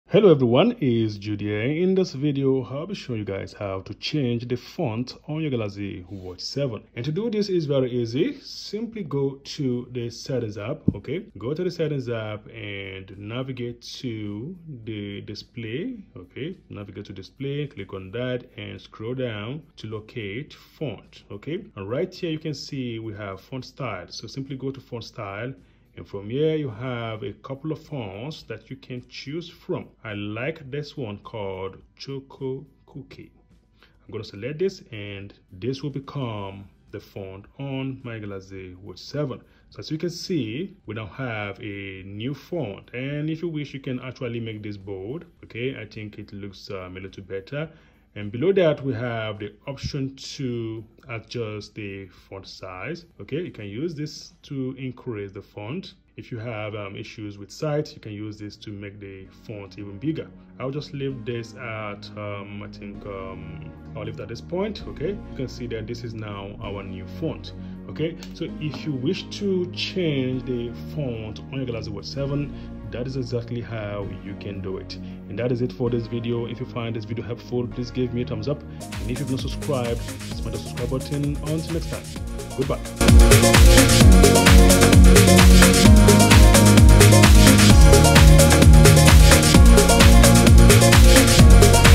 How to Change Font on Galaxy Watch 7
This video shows how you can change the font of your Galaxy Watch 7. So if you want to increase font size or use gestures to control your Galaxy Watch 7, this video shows exactly how to do that.